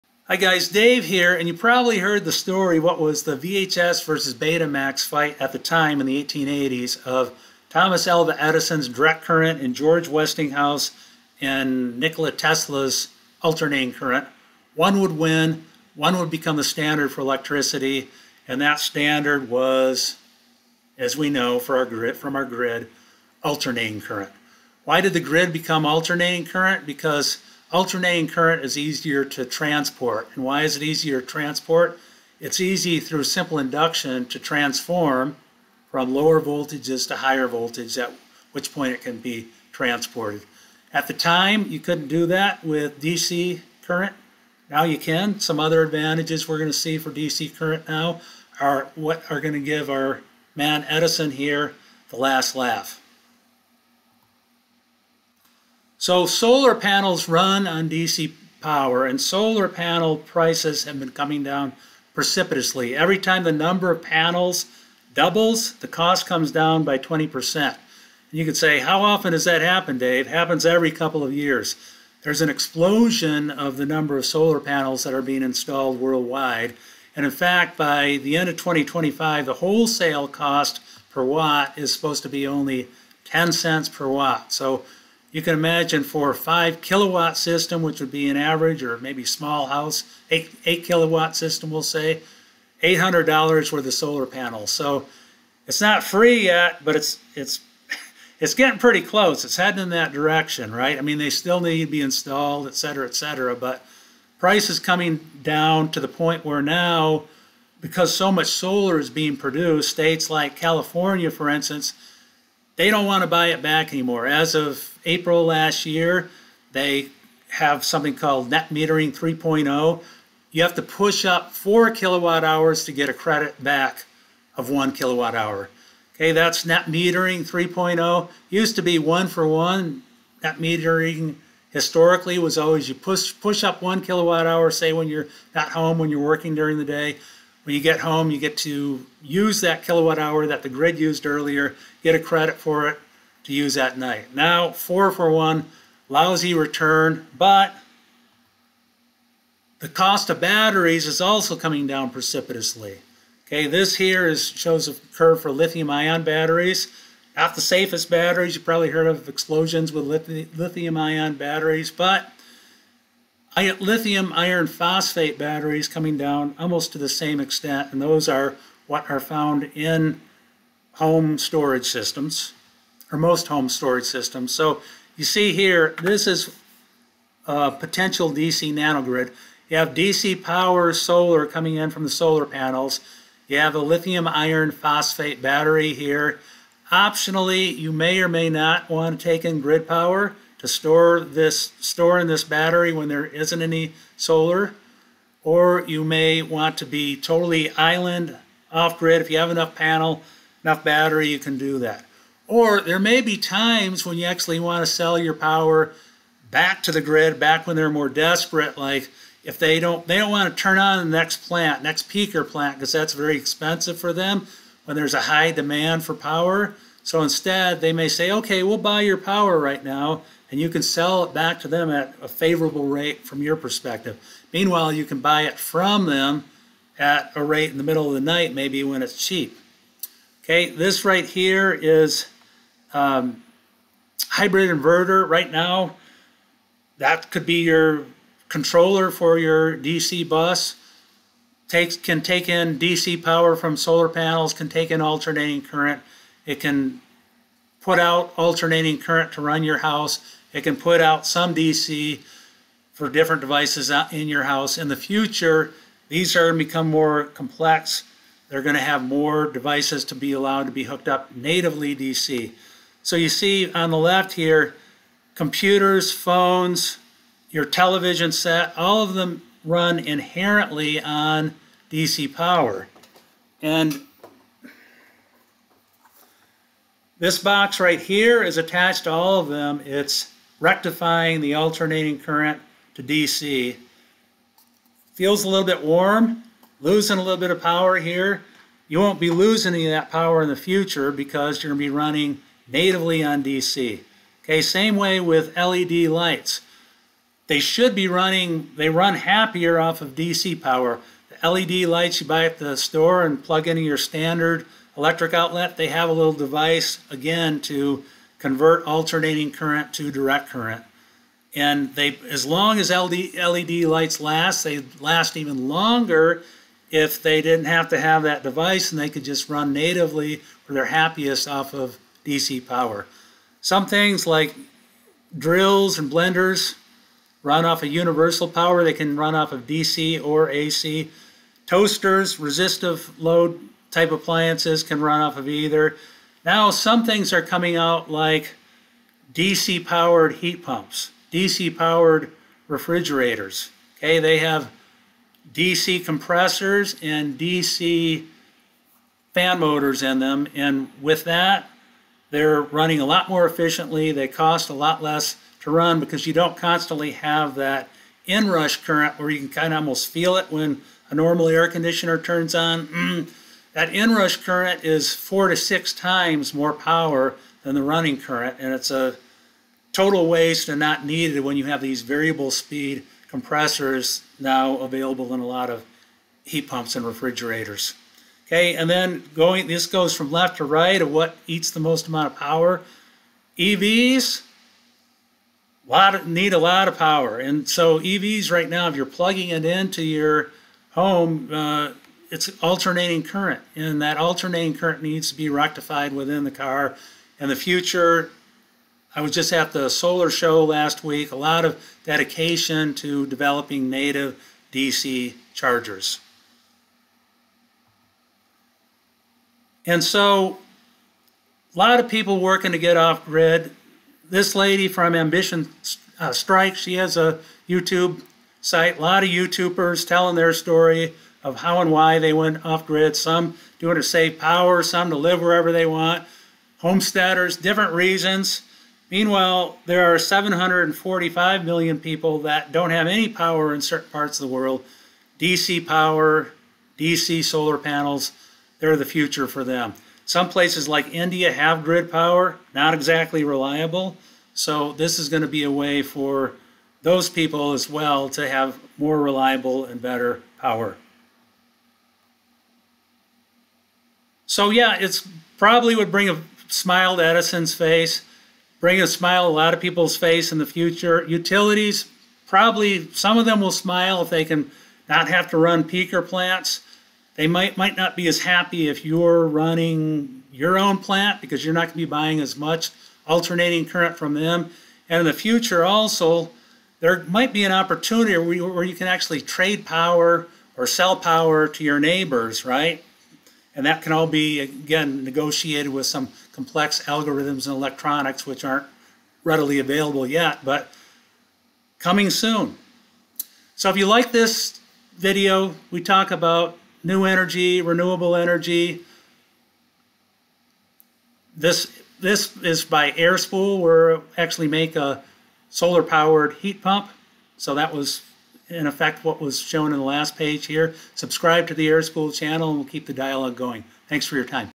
DC nanogrids: Why and how DC power will become the new AC power for your home.
Edison lost the last round of the energy wars to Tesla and Westinghouse. But, with solar (i.e., DC) power almost free these days, we will soon run our own DC nanogrids to energize our homes. Why? Because almost everything we use runs on DC now, and because the cost of that DC power is approaching zero.